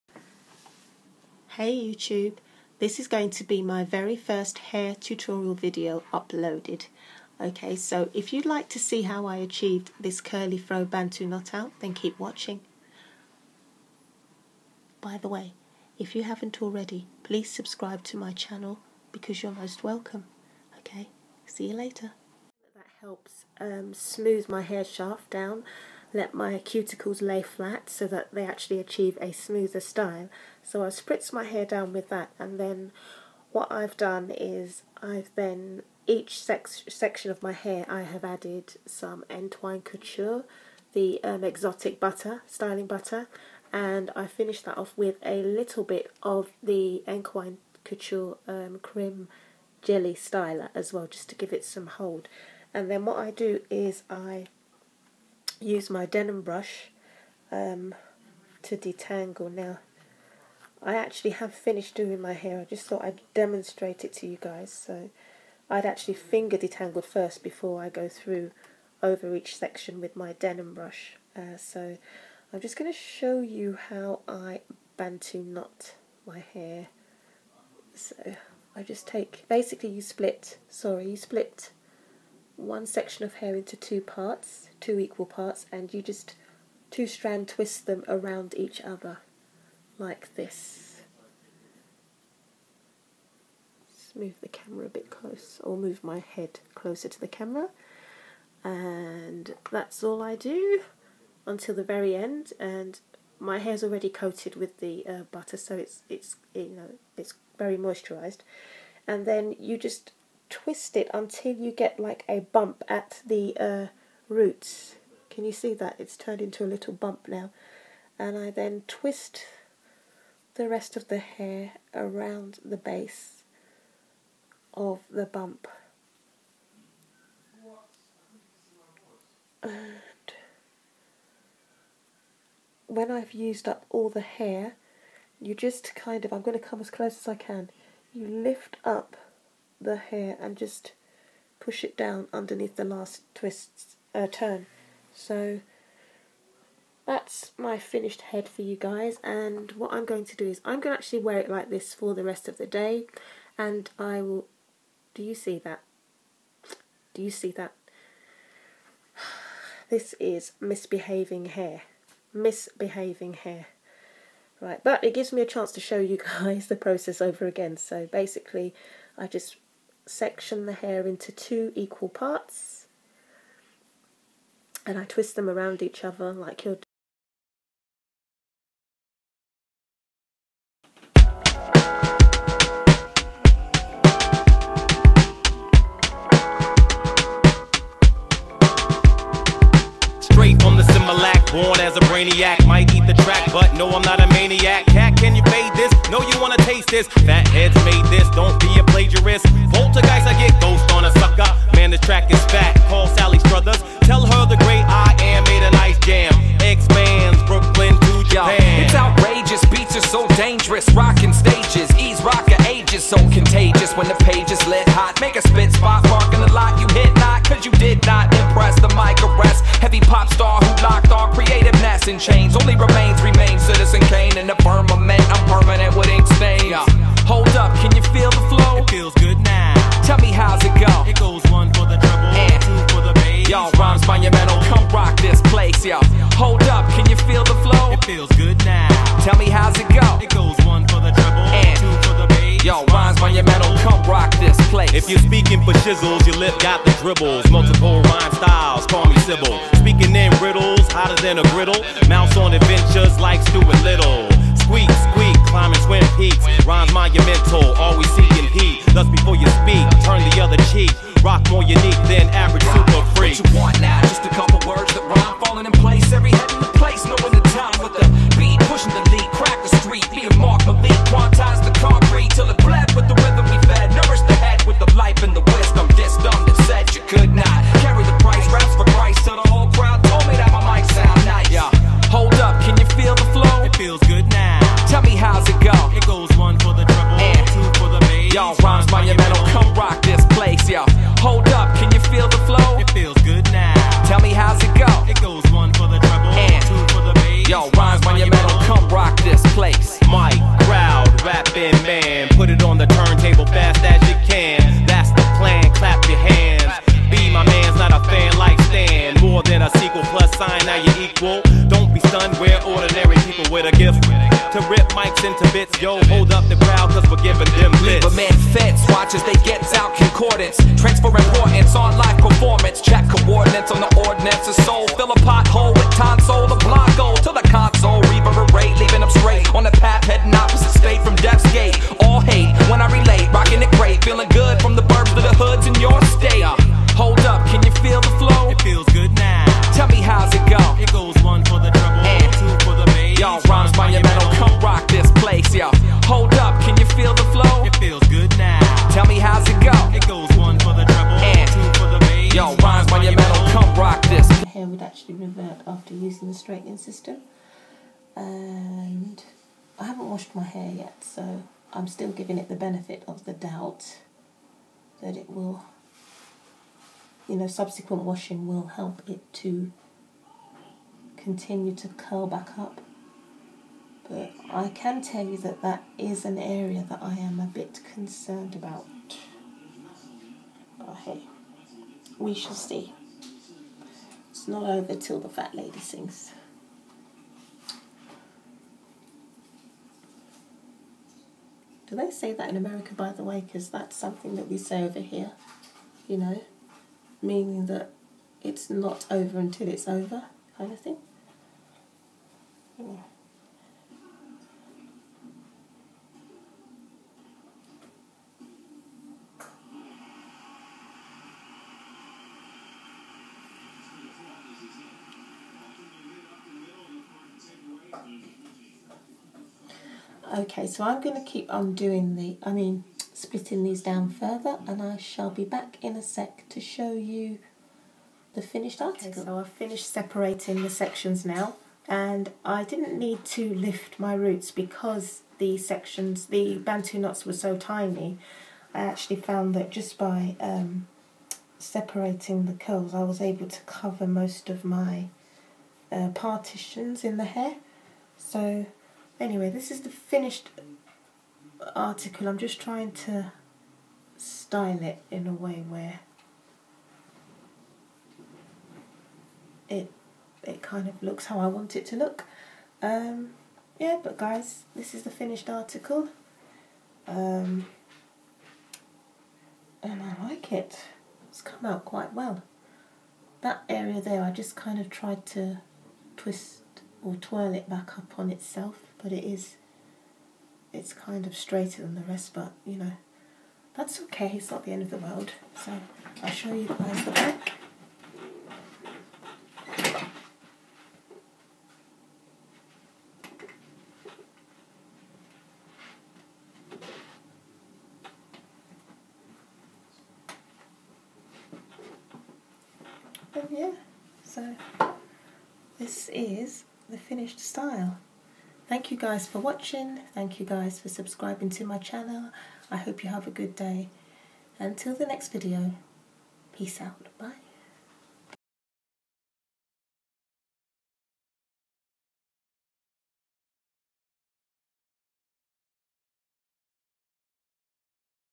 HOW TO: Get a frizz free Bantu knot out!!
Hi All,
In this video I am attempting my very first tutorial. Please leave any constructive comments below. I love Bantu knot outs but mine have a habit of being infested with frizz. Well in this video I seem to have been able to conquer those annoying fuzzy bits we all loathe!
Happy viewing and thank you for subbing. Stay Blessed xxx

I'm on Instagram @afro4t

Products used;
Aloe vera mix (1 part aloe juice mixed with 4 parts water)
Entwine Couture Exotique Butter'
Entwine Couture Creme Jelly Styler.
35 ounces of the patience of Job Lol!

TIPS:
moisten stretched hair do not over-spray
note direction of twist and take down in the opposite direction
sleep in a silk/satin scarf,  bonnet or use a pillowcase
ensure hair is completely dry before taking the twists down
use your favorite hair oil on your hands before you begin take down
don't rush
Enjoy the results! xxxxx